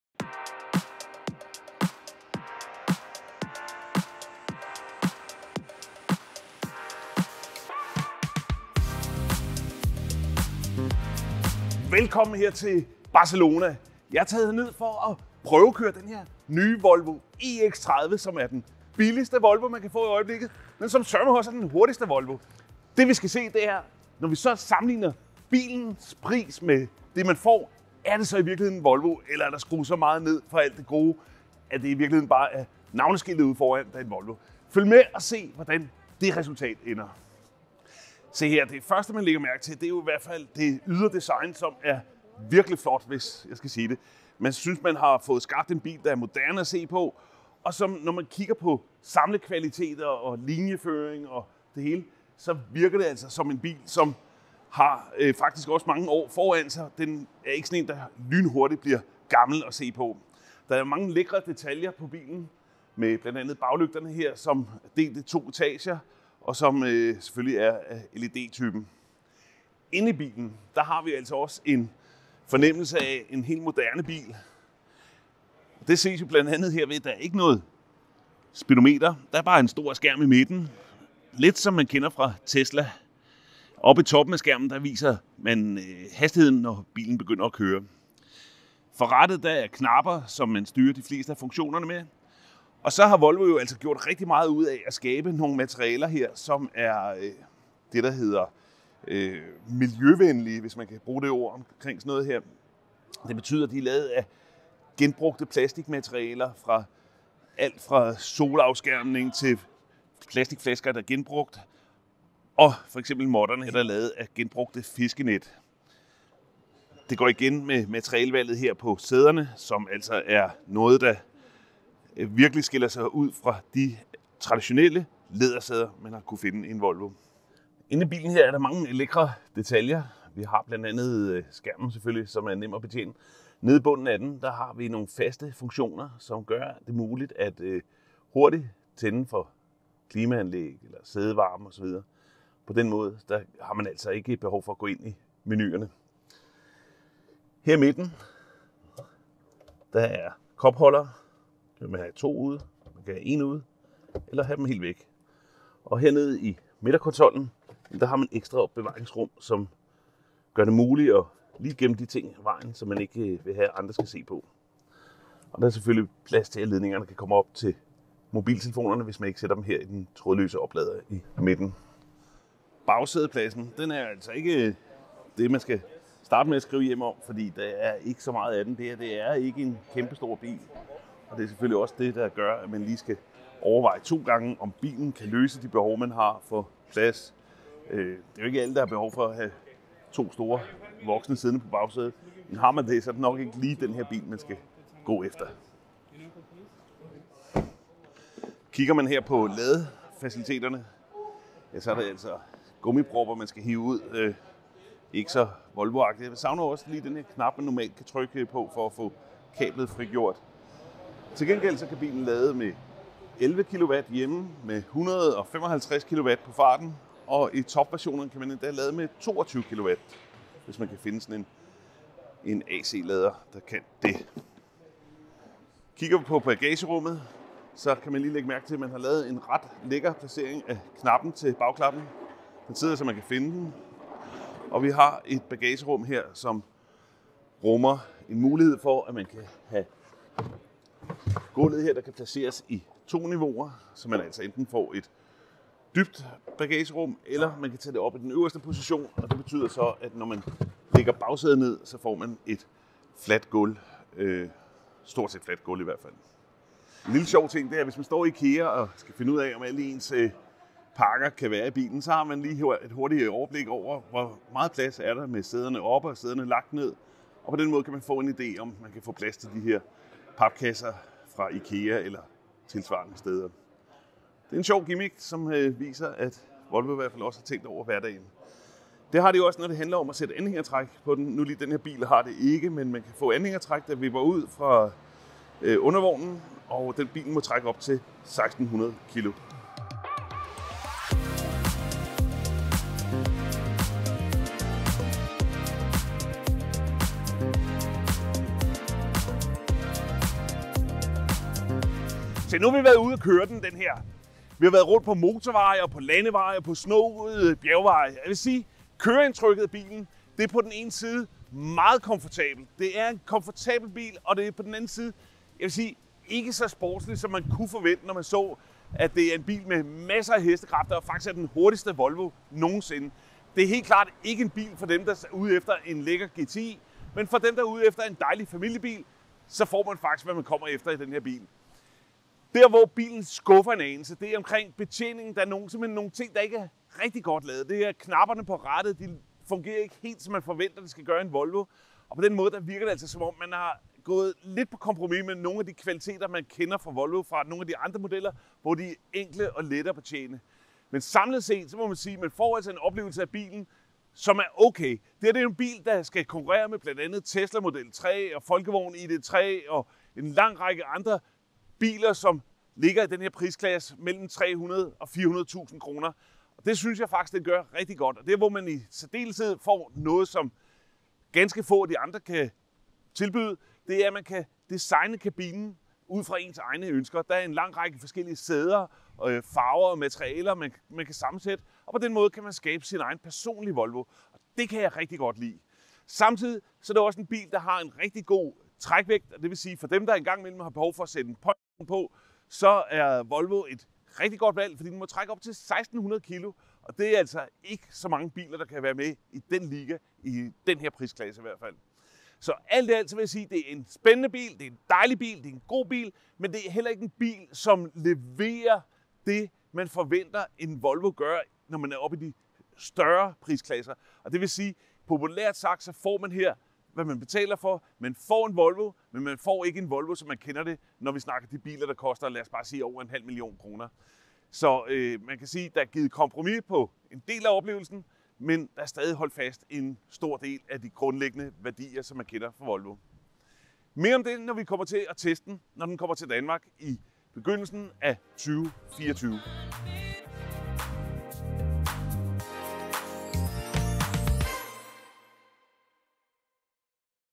Forpremiere: Volvo EX30
Turen går til Barcelona, hvor vi for første gang for lov at køre en tur i den nye, meget hypede Volvo EX30. Elbilen er det svenske mærkes billige elbil og kommer med en fra-pris på 274.000 kr. Den mest interessante udgave er dog EX30 Plus i den baghjulstrukne udgave. Den kommer med en pris på 337.000 kr., og har det store batteri og dermed en rækkevidde på 476 km.

Indhold i videoen:
0:00 Intro
0:17 Bilens data
0:37 Eksteriør
1:13 Forsæde
2:52 Bagsæde
3:20 Opladning
3:49 Bagagerum
4:38 Anhængertræk
5:02 Konklusion